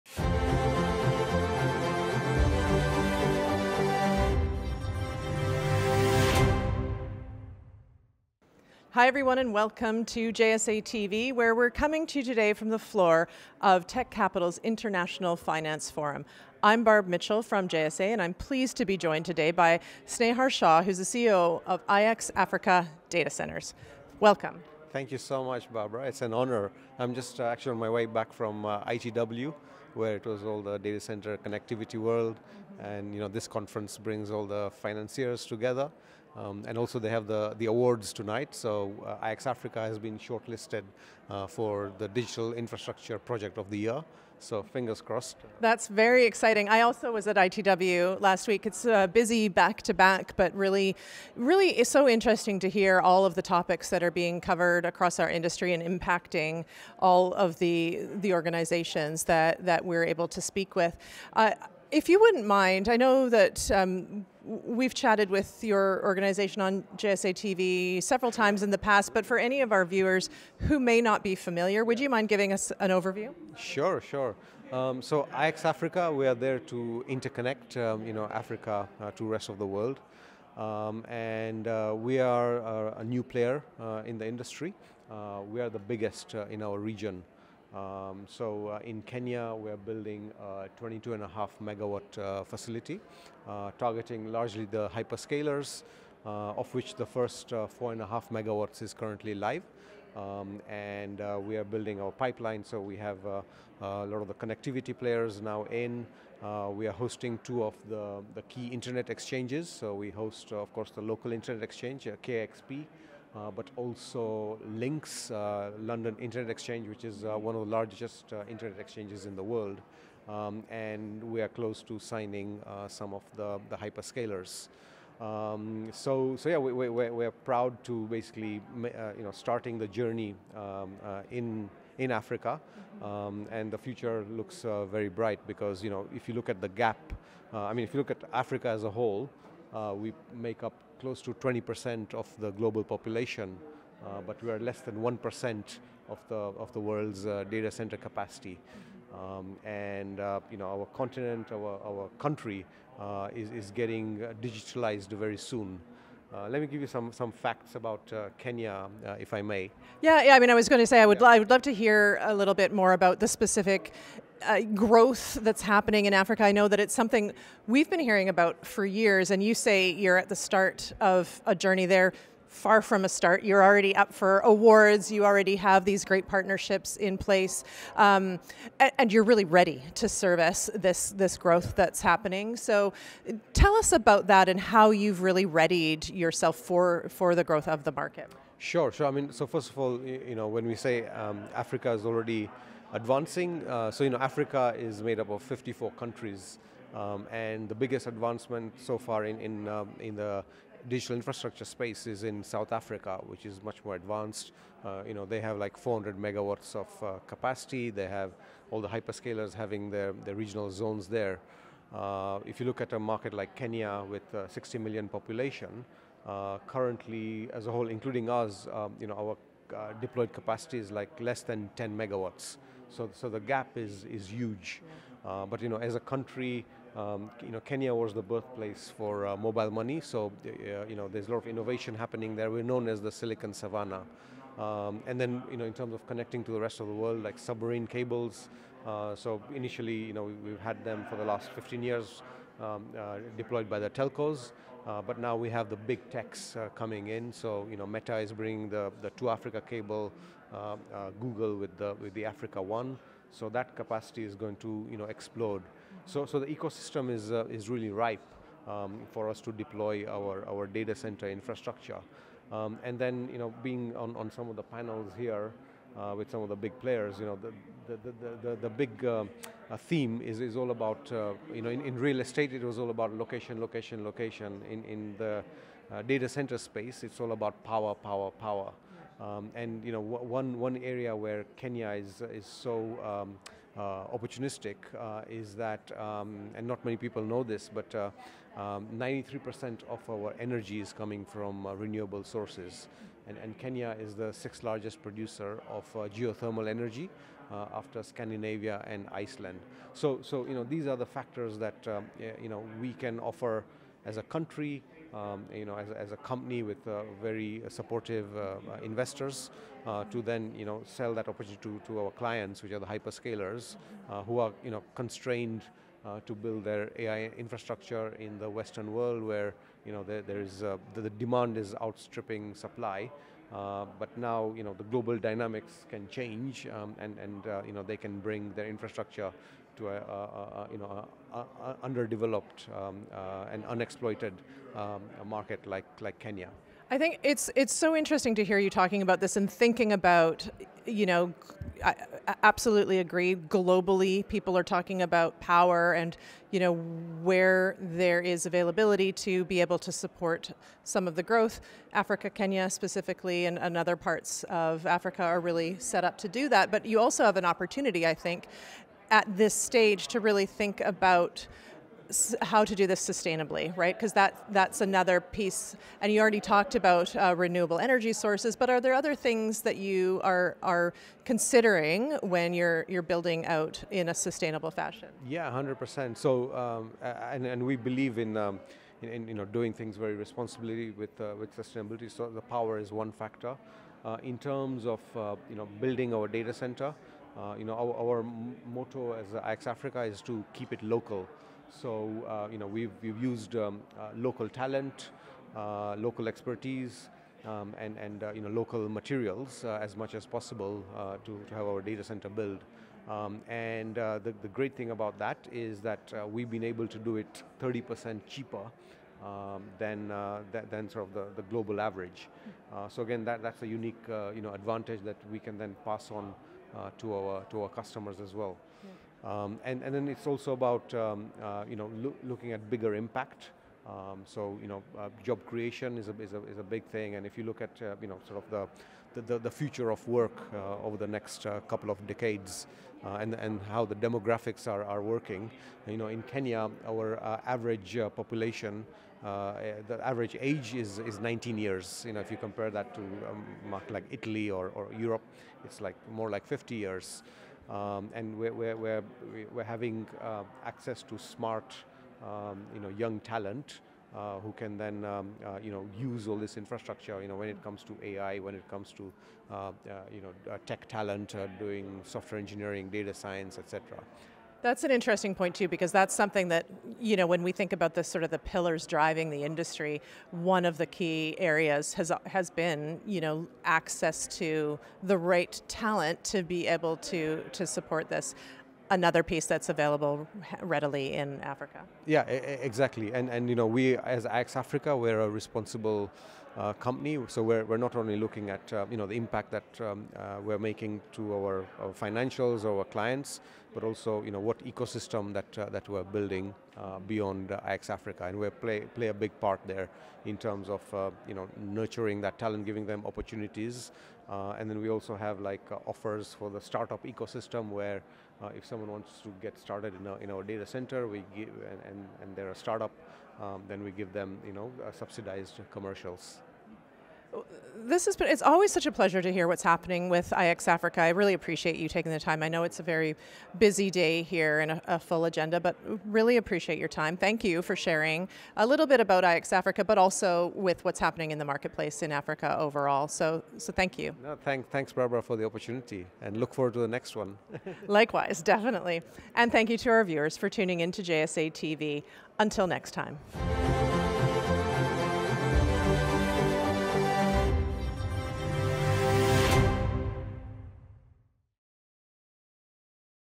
Snehar Shah with IX Africa Data Centre | The Tech Capital: International Finance Forum 2025
Join JSATV at the Tech Capital: International Finance Forum 2025 as Snehar Shah, CEO of iXAfrica Data Centres, shares how Kenya is becoming a key player in digital infrastructure.

Learn about their 22.5MW campus, renewable energy edge (93%!), and how iXAfrica Data Centres supports hyperscalers, startups, and local talent.